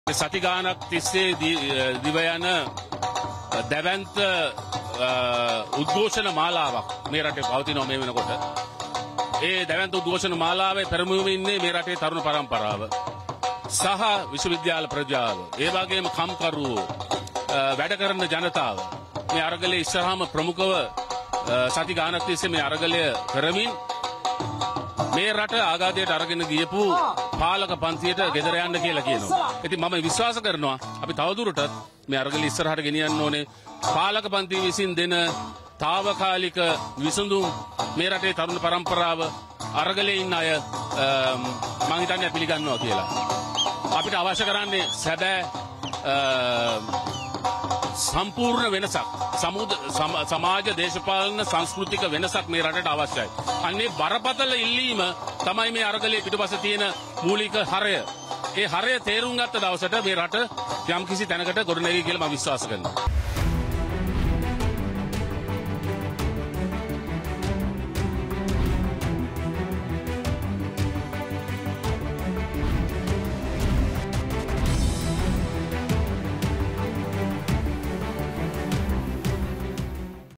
මහාචාර්ය සෞම්‍යත් ජන අරගලයේ | 2022.04.30
රළු ආණ්ඩු විරෝධයක් සමගින් මහාචාර්ය සෞම්‍යත් ජන අරගලයේ සිට...